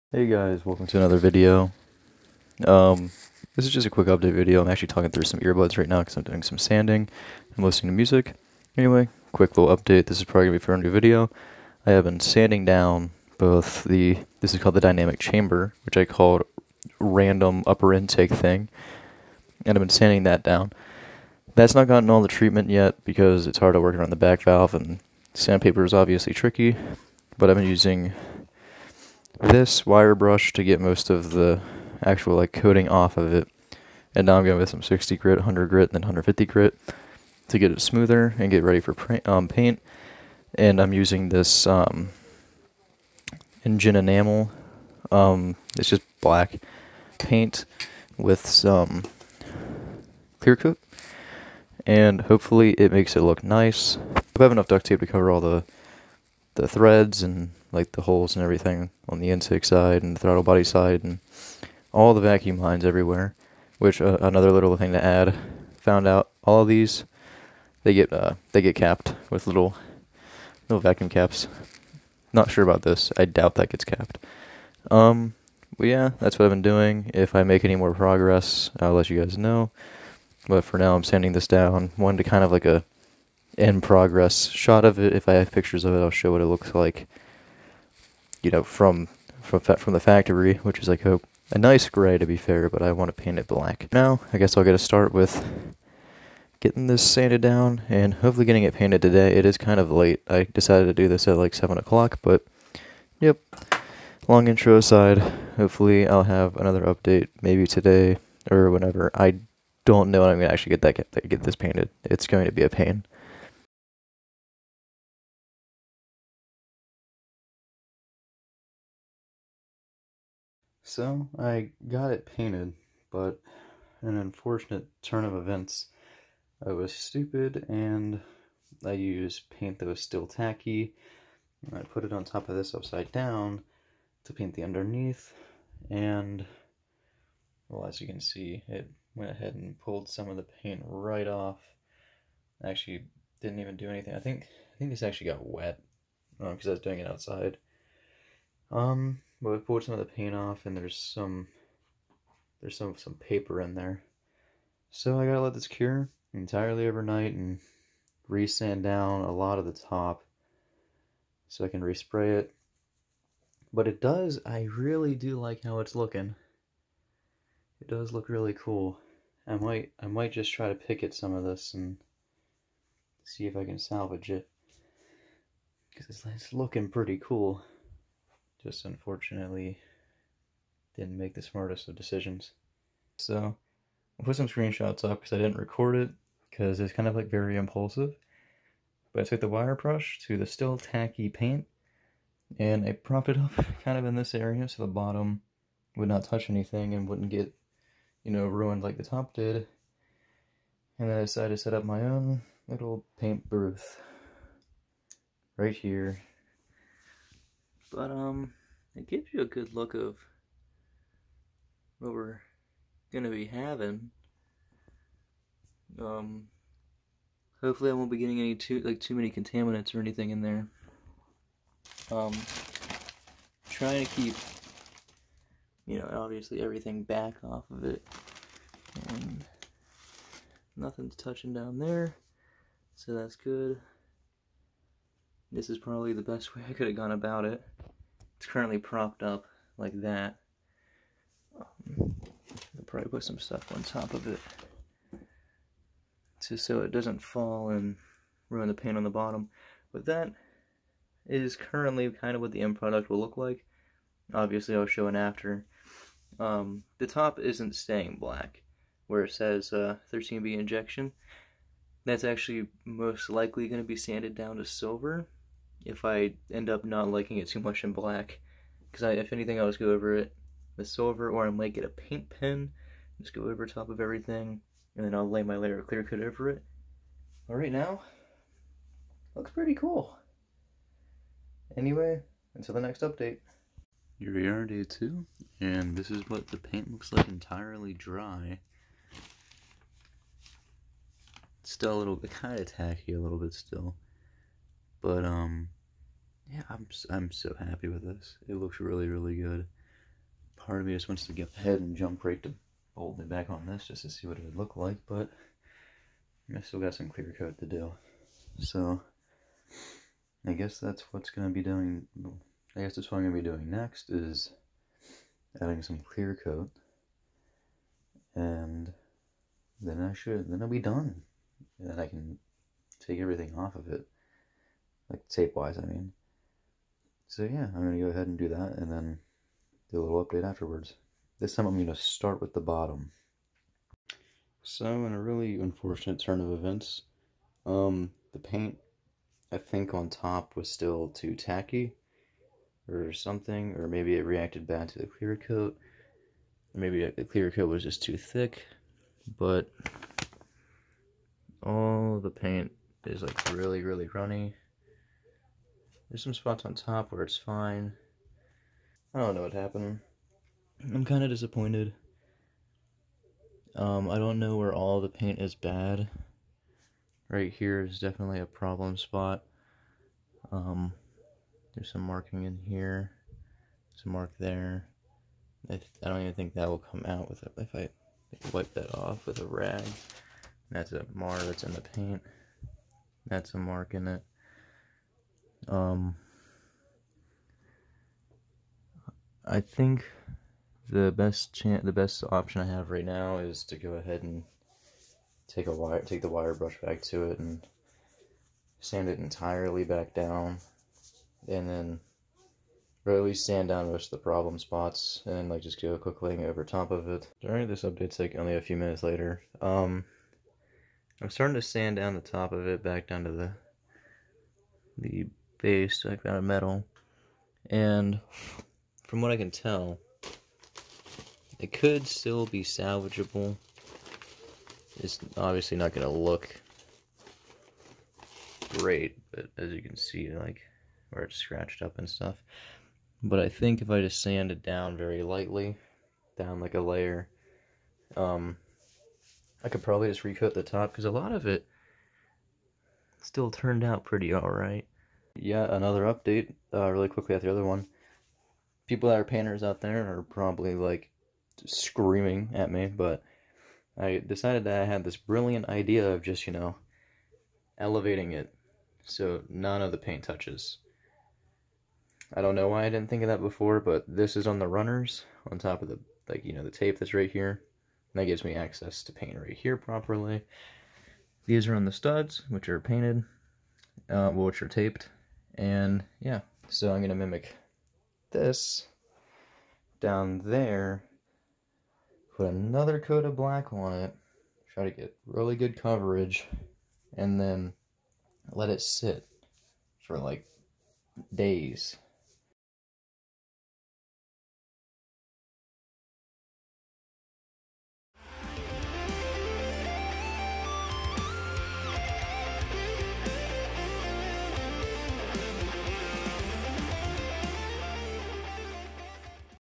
Painting My Intake Manifold! (FC Revival Pt:2)
In today's video, I attempt, and fail, at painting my intake manifold! Join me on my journey and how I learn from my mistakes